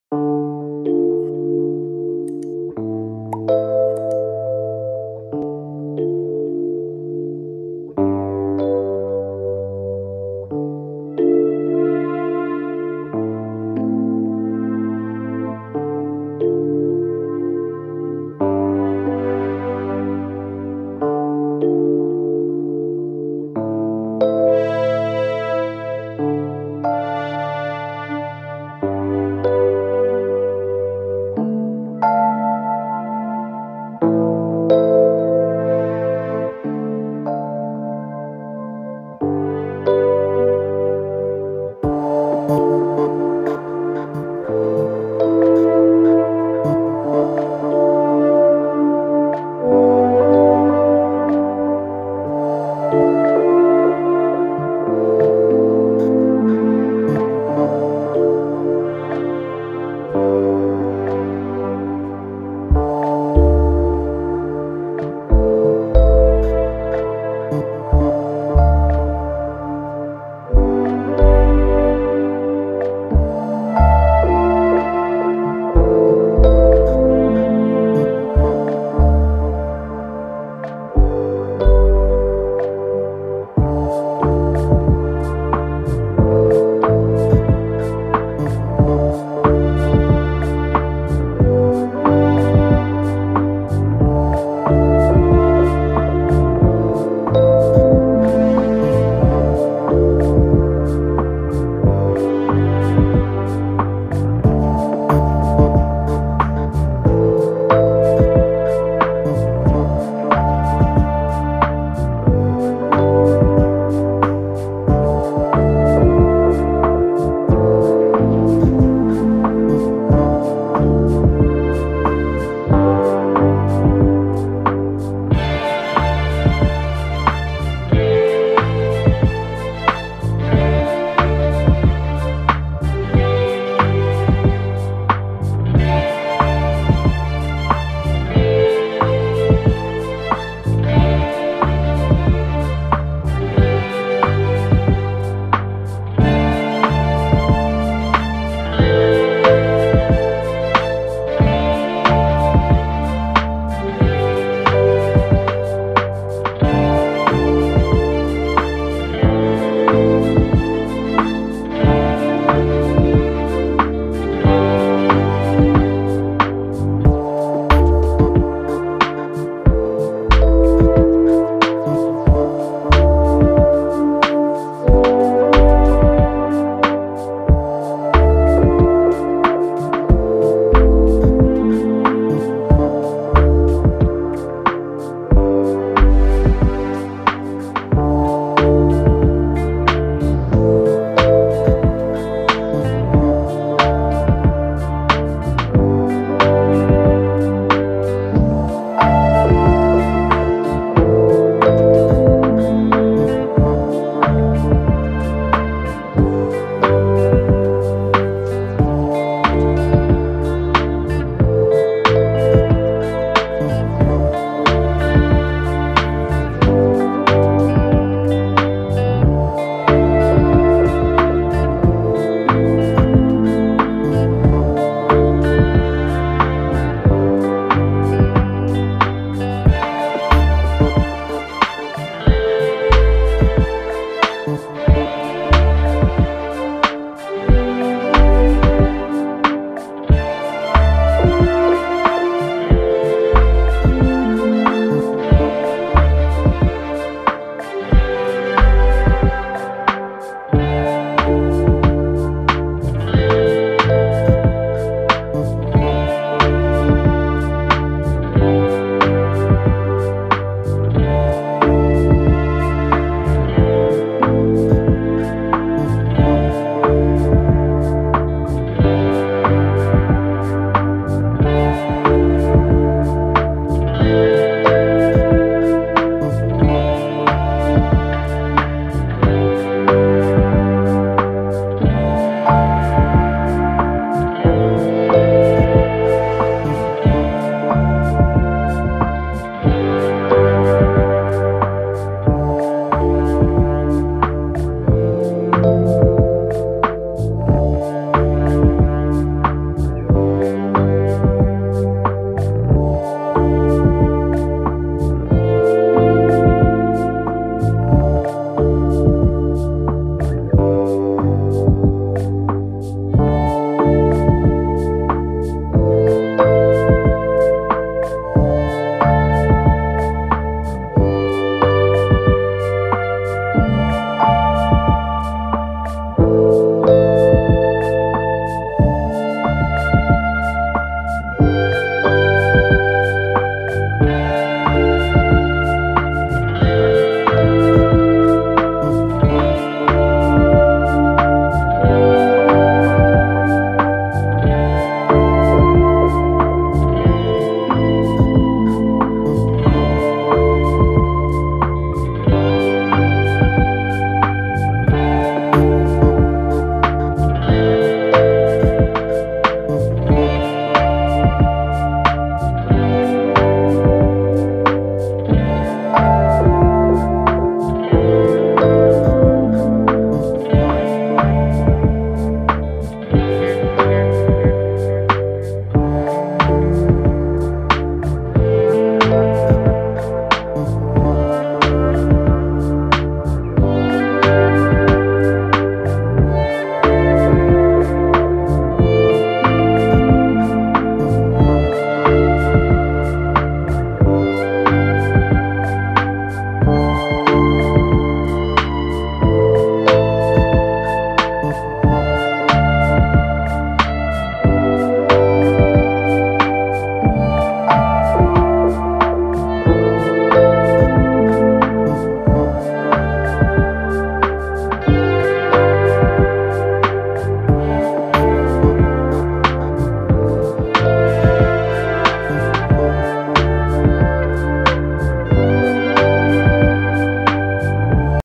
Musical instruments to light up your mood
Musical instruments payed together to cheer you up during the day and lighten up your mood. Enjoy

Cheers.